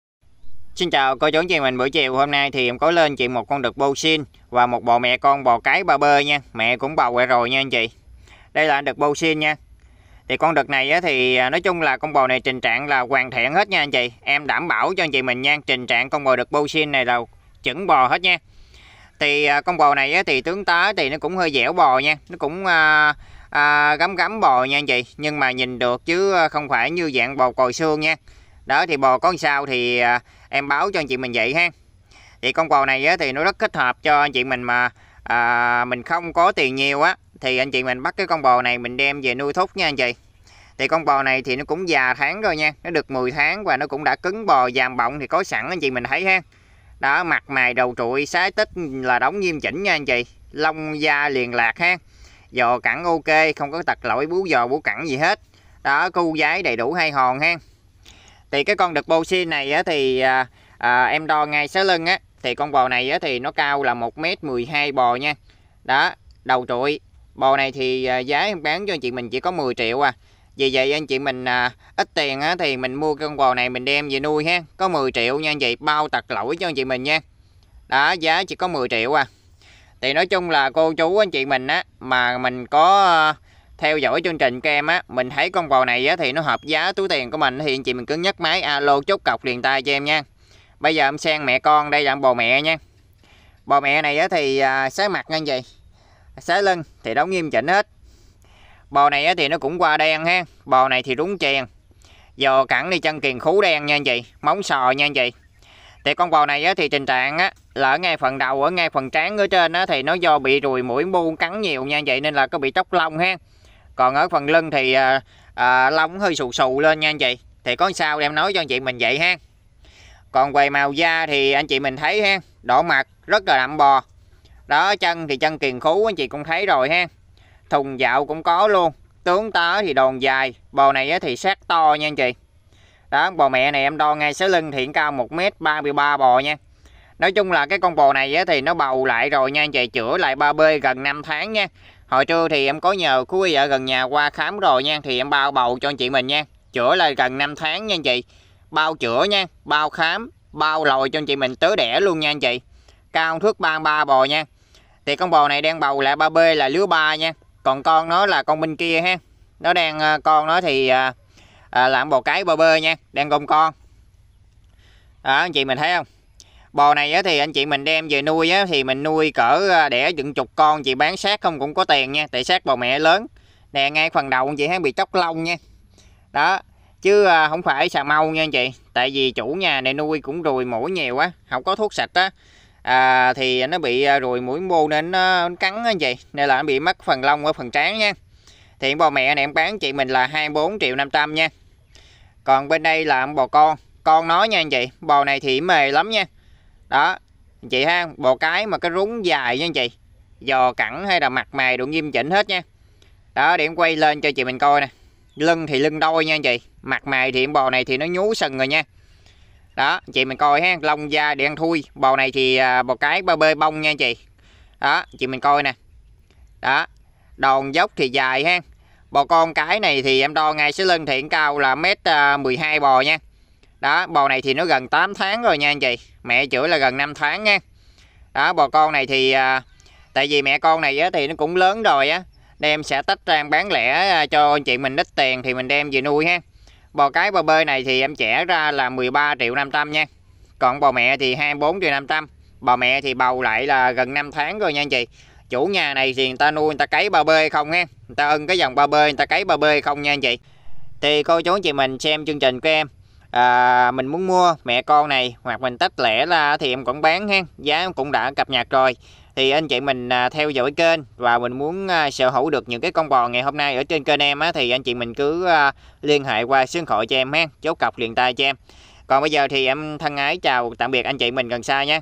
15/3 Mới Về Đực Sind 10tr. Bộ Mẹ Con Cái 3B Bầu Gần 5 Tháng ( Tách Mẹ 24,5tr/ Cái 3B 13,5tr )
Giá bò hôm nay
Đực sind 10tr. Mẹ con cái 3b ..

Ngân Hàng Quân Đội MB Bank STK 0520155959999
Tên Nguyễn Văn Phúc